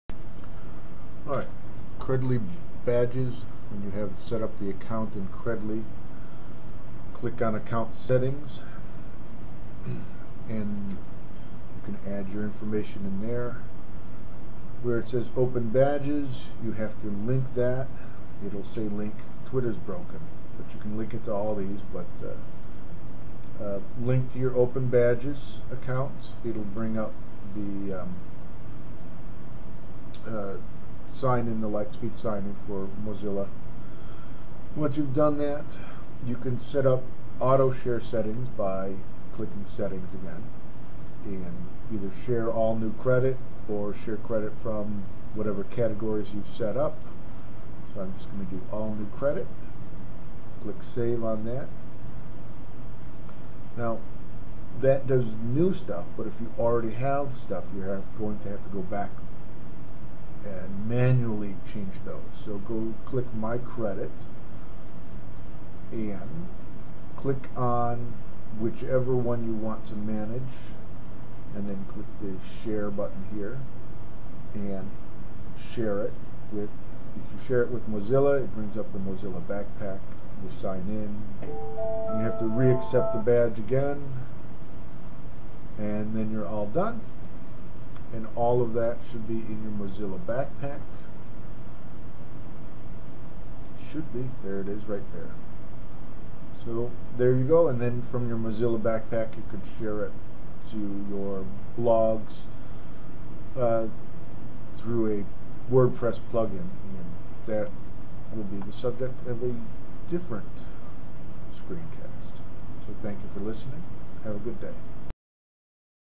Sharing credly badges with Mozilla backpack
How to chare the badges you have earned with Mozilla backpack